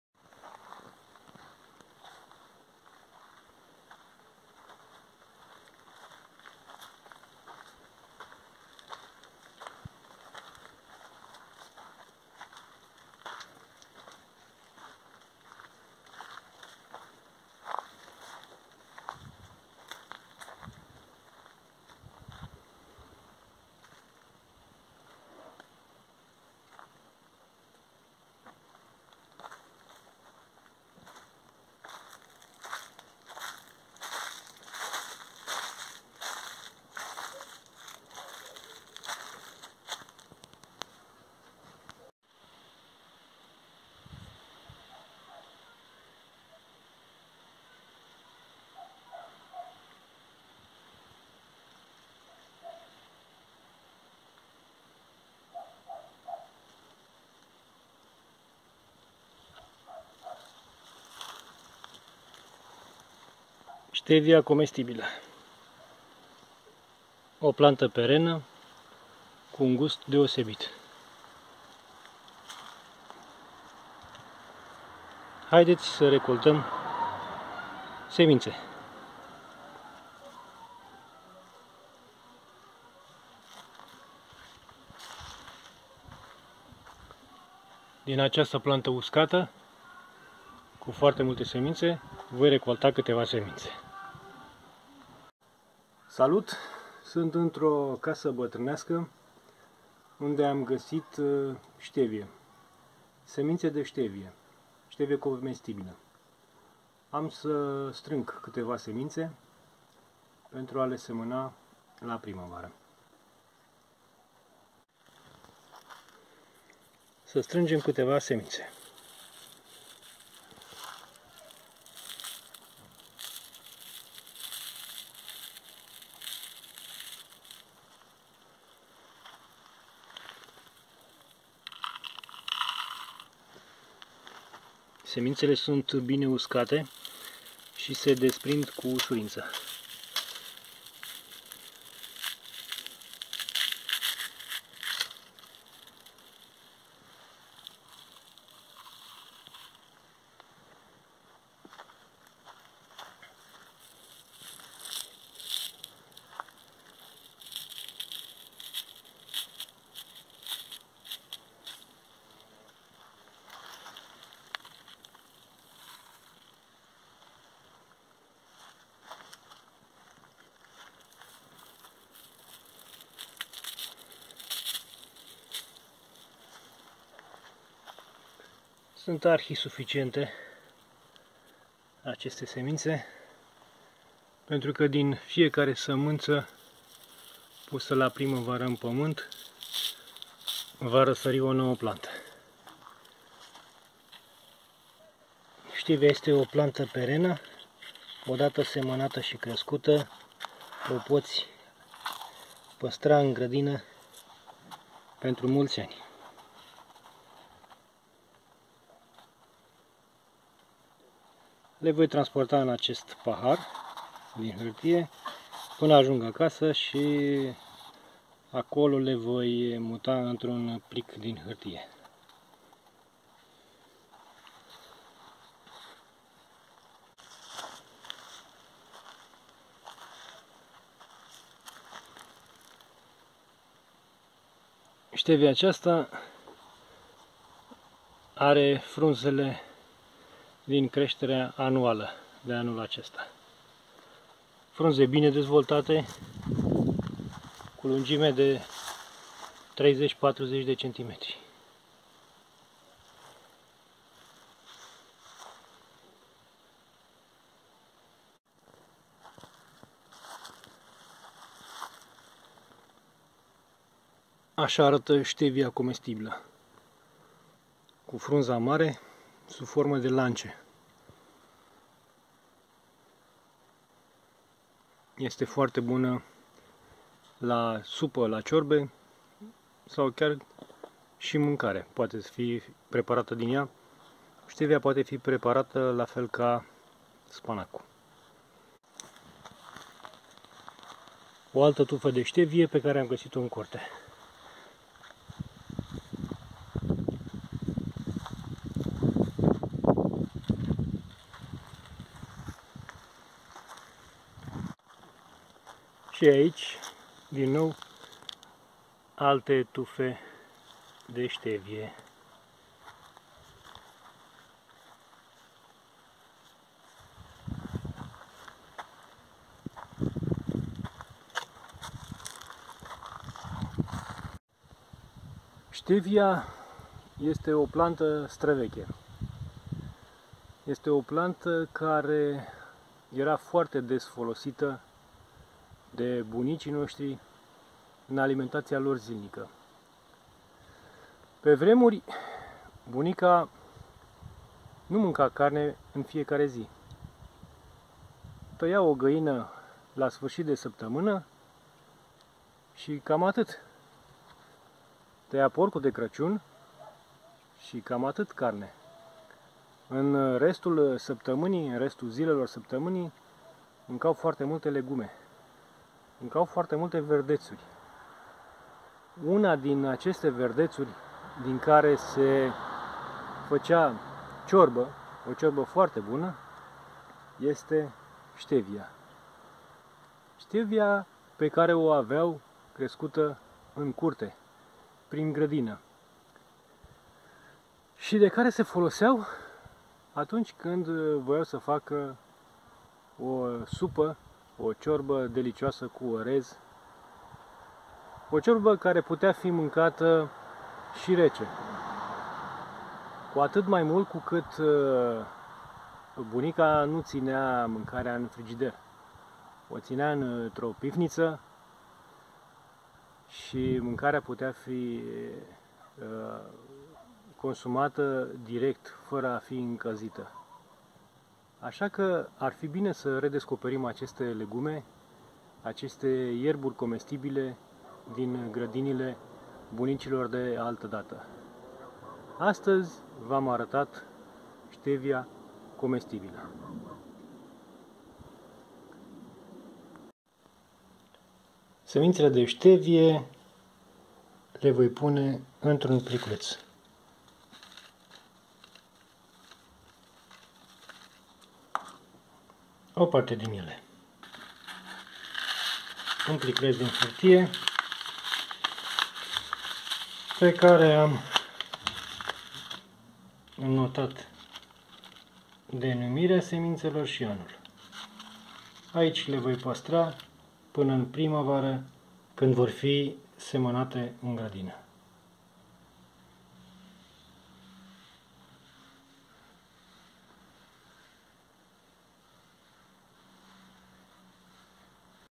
Ștevia- recoltarea semințelor
Ștevia, o plantă perenă deosebit de hrănitoare și de comună dar cam uitată. O plantă care creștea cândva în fiecare curte și constituia o bază pentru mâncarea de vară.